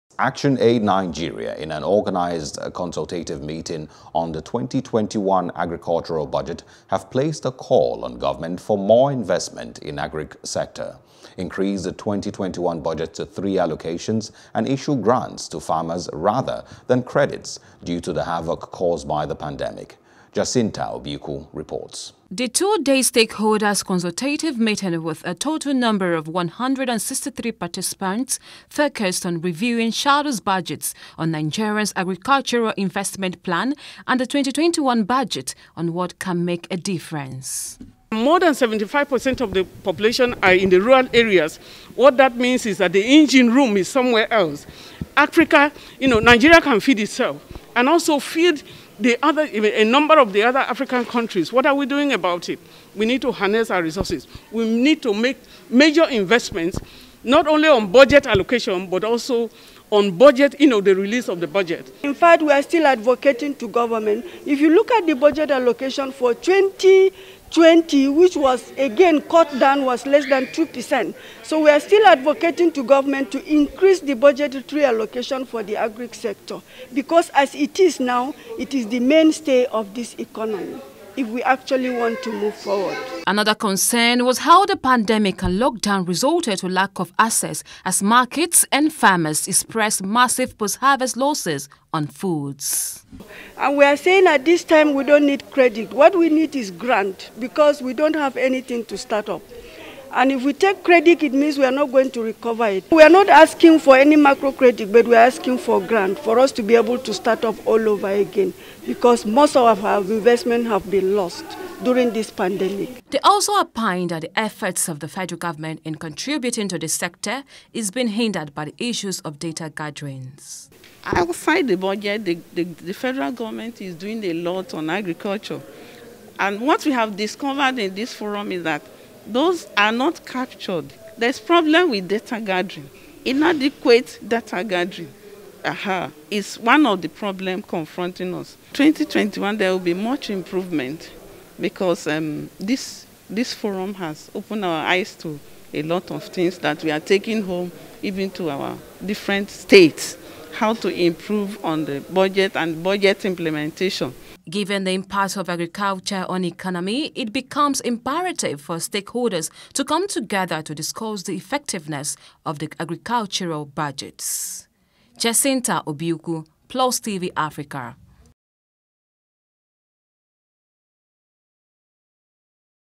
Agriculture Budget: Stakeholders Urge Government For Allocations And Grants (NEWS | BUSINESS)
Actionaid Nigeria in an organised consultative meeting on the 2021 Agricultural Budget has placed a call on government for more investment in Agric sector, increase the 2021 budget to three allocations and issue grants to farmers rather than credits due to the havoc caused by the pandemic.